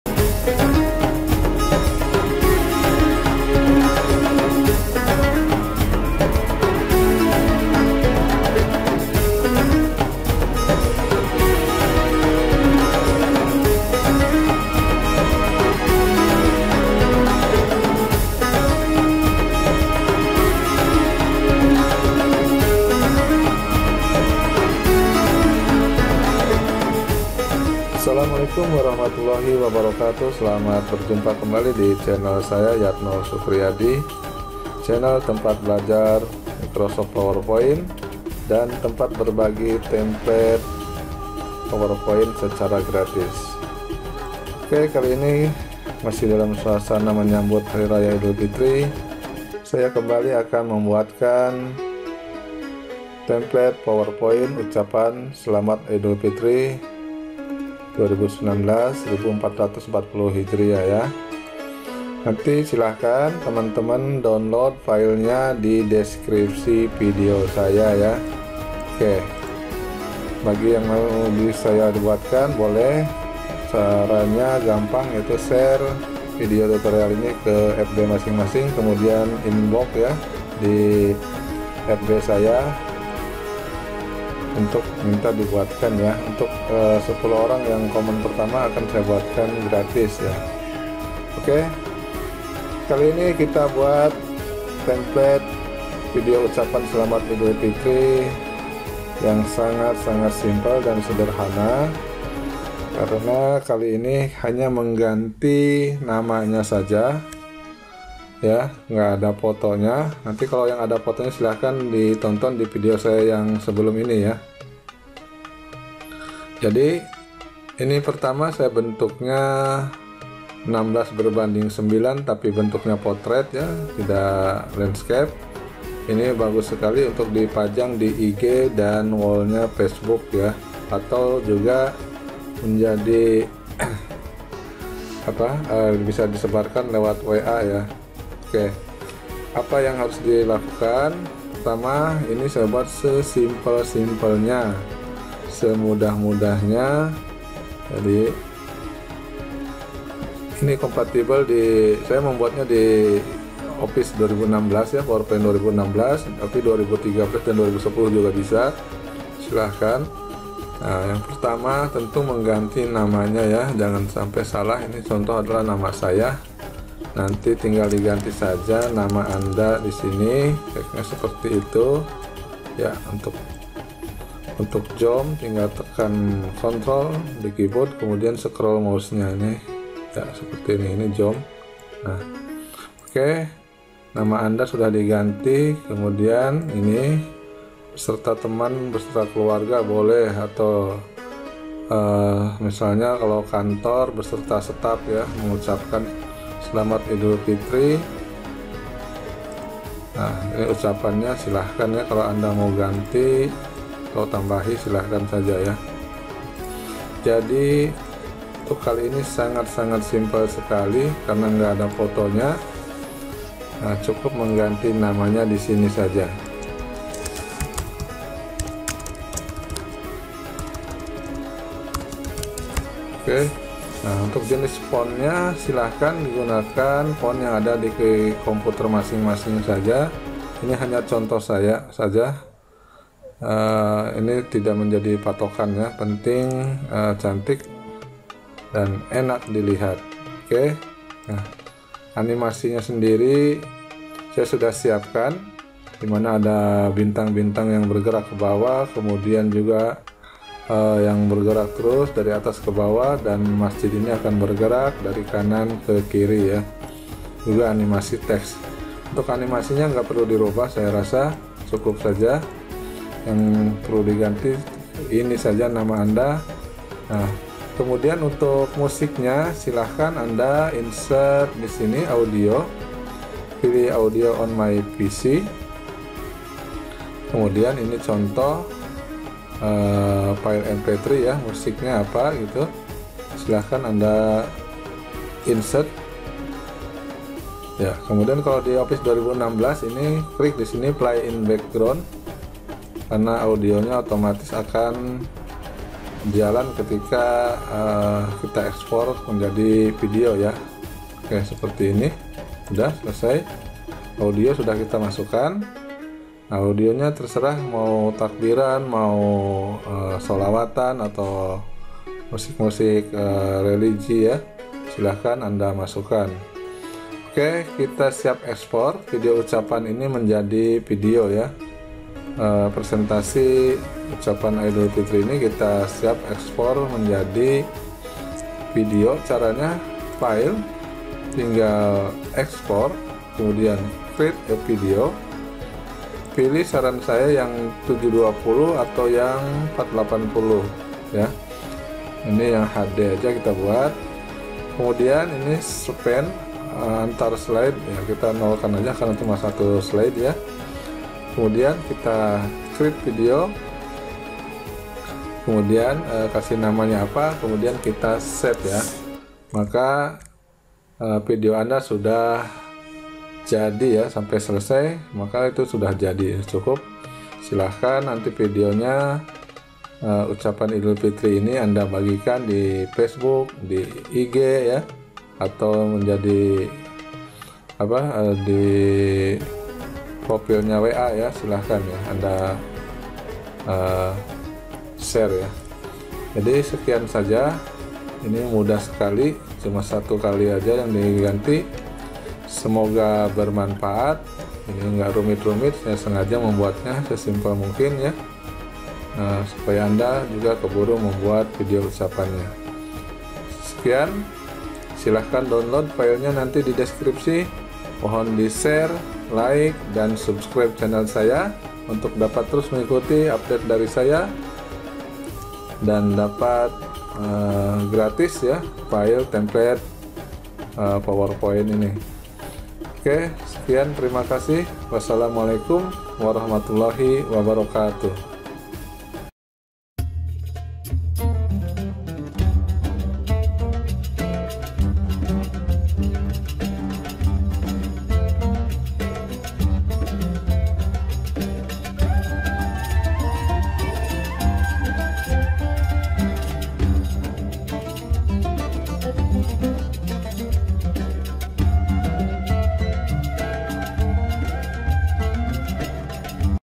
GRATIS Template Ppt, Ucapan Selamat Hari Raya Idul Fitri 2019 | 1440 H
Saya buatkan template power point untuk Ucapan Selamat Hari Raya Idul Fitri 2019 / 1440 H ini sangat simple sekali , untuk memudahkan teman-teman mengedit nya, kali ini hanya mengganti teks untuk nama dan memasukan musik kedalam template video ini.

Silahkan download templat power point untuk membuat video ucapan Idul Fitri 2019 di :

Bantu SHARE ya, biar teman2 nya bisa membuat juga video ucapan yang keren ini

Semoga Bermanfaat
Berbagi Tak Pernah Rugi
Hidup Harus Jadi Solusi